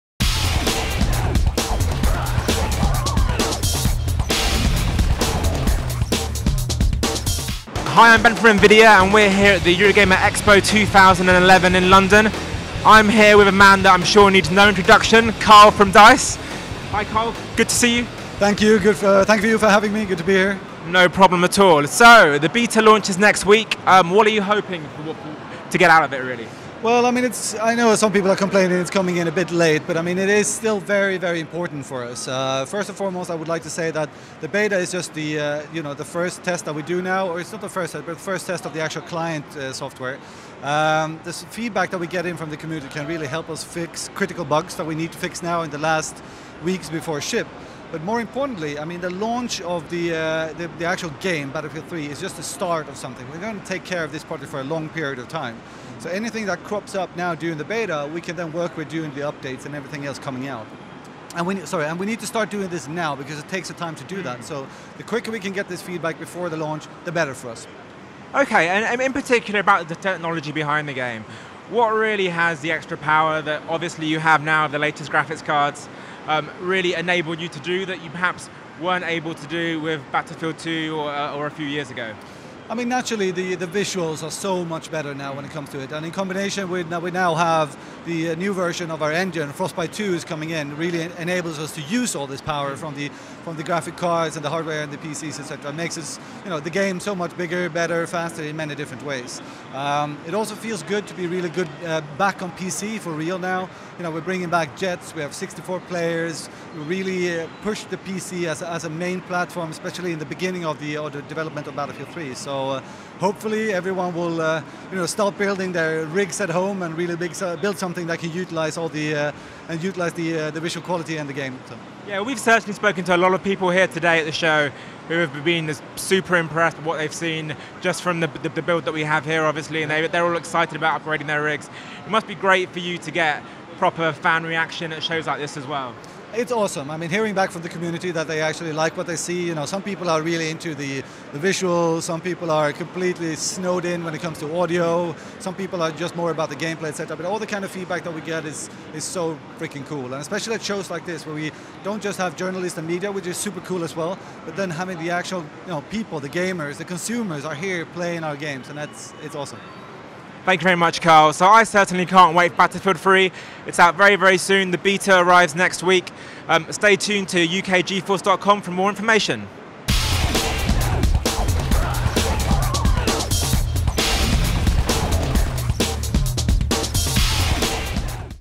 Battlefield 3 EuroGamer Expo 2011 Karl-Magnus Troedsson Interview
GeForce UK chats to Karl-Magnus Troedsson, the General Manager of DICE, at EuroGamer Expo 2011 at Earl Courts, London, England.